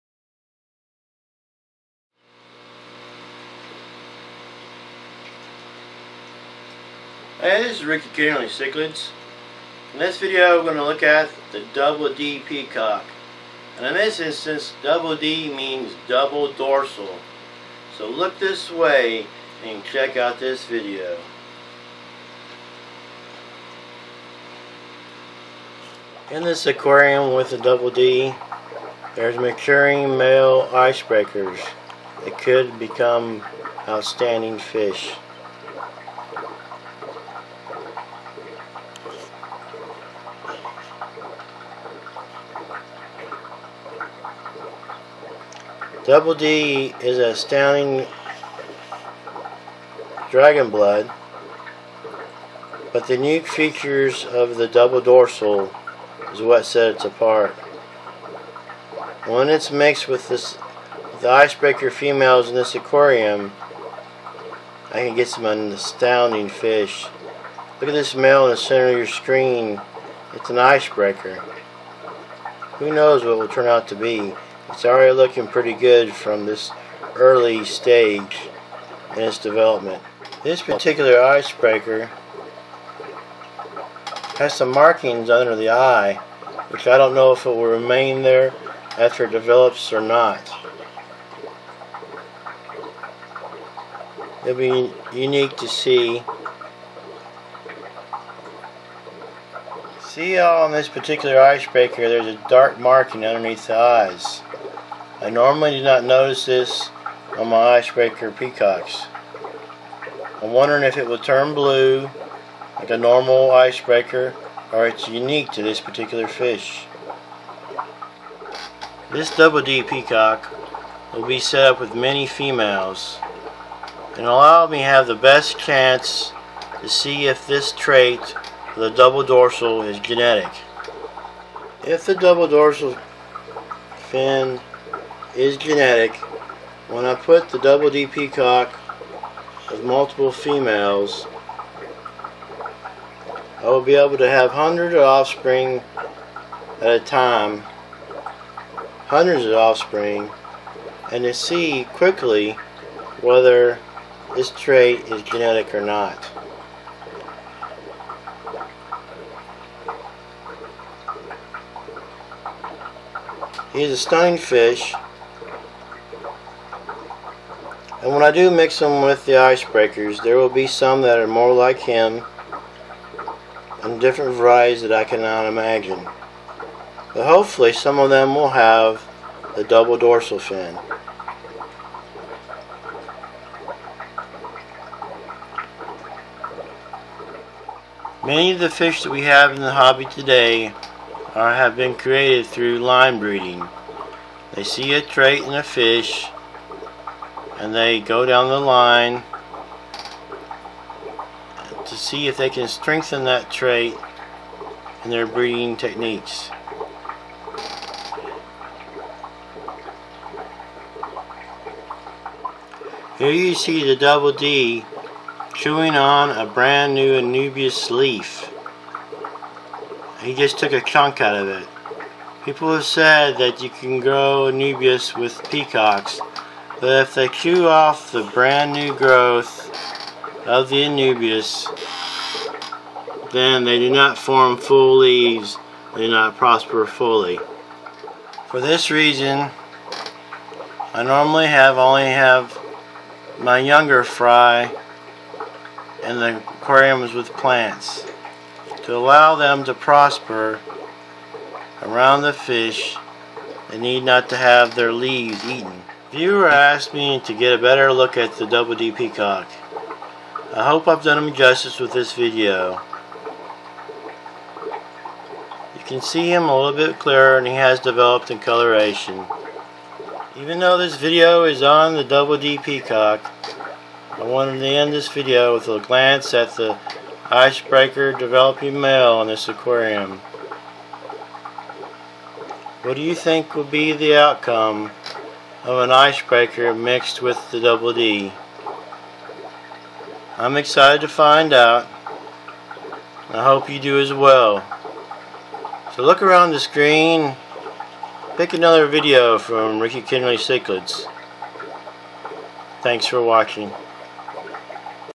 The Double Dorsal Fin African Peacock Cichlid
This video is on the Double Dorsal Fin African Peacock Cichlid.  It is a dragonblood peacock cichlid that has a double dorsal fin.